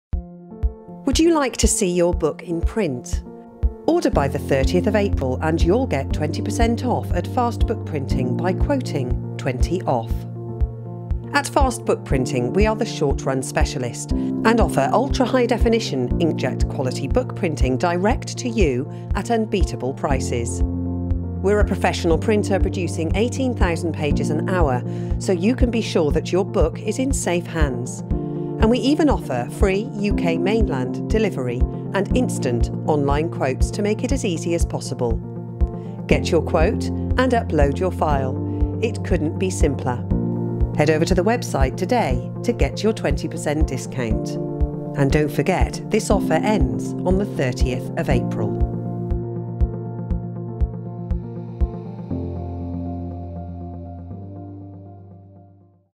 Get 20% off at Fast Book Printing
This advert was created to advertise a discount code that was available at Fast Book Printing ands was shared across social media to drive traffic to the site.